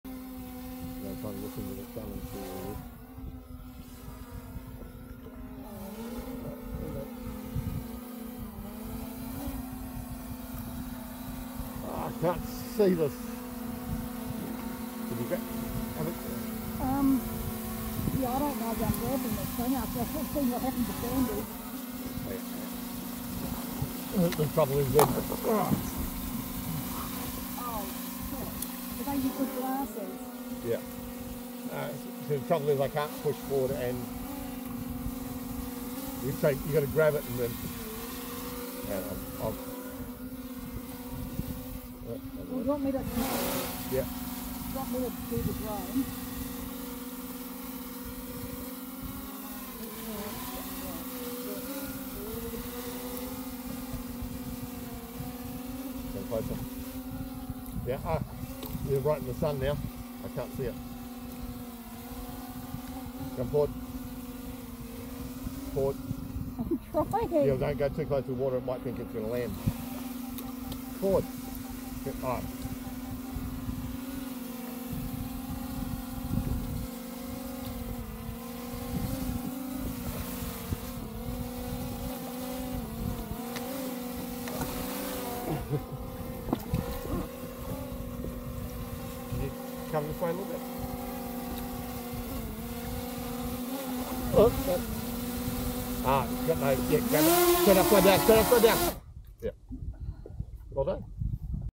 Boat Challenge - catch the drone.
We finally took our Inflatable out, and launched the DJI Air 3 Pro drone.  Retrieving it back into the boat while drifting was a little challenging.